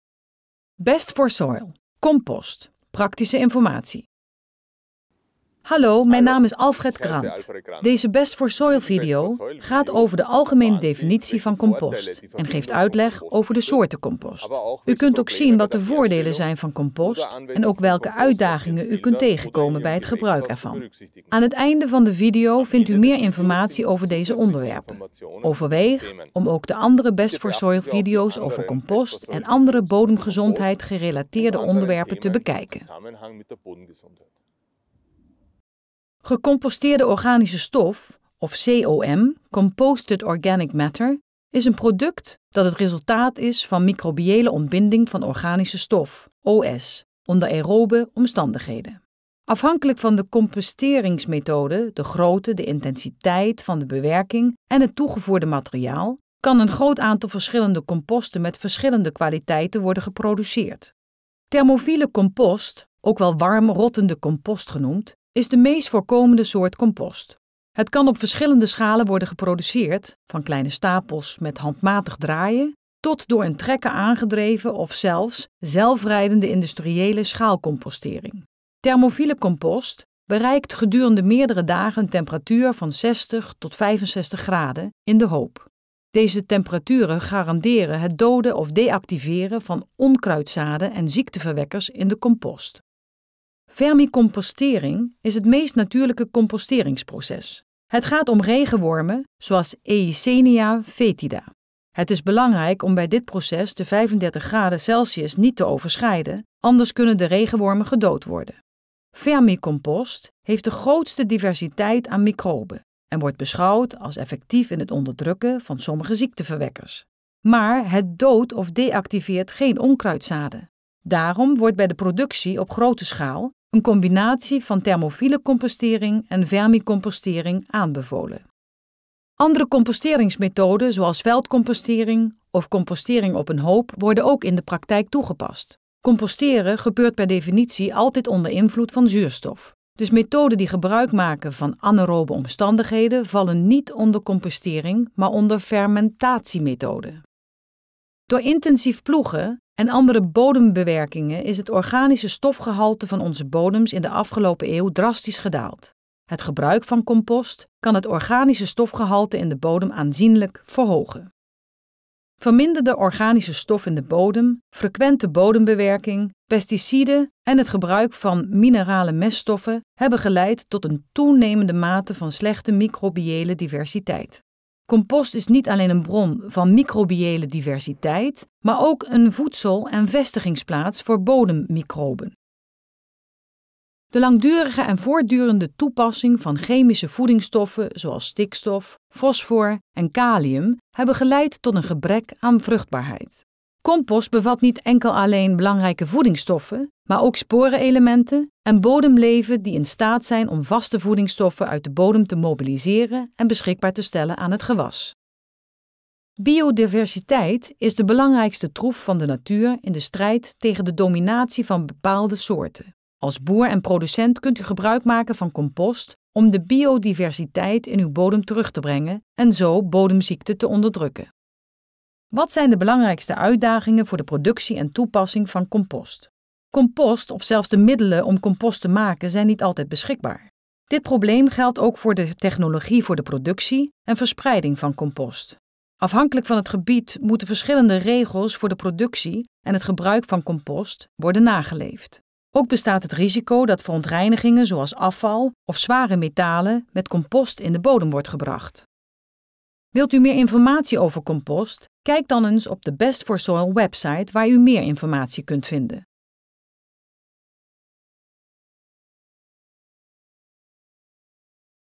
Compost algemene informatie
Deze Best4Soil video vertelt u over de algemene definitie van compost en de soorten compost. Ook kunt u zien wat de voordelen zijn en welke uitdagingen u moet aangaan bij het gebruik van compost. Aan het einde van de video vindt u aanvullende informatie over deze onderwerpen.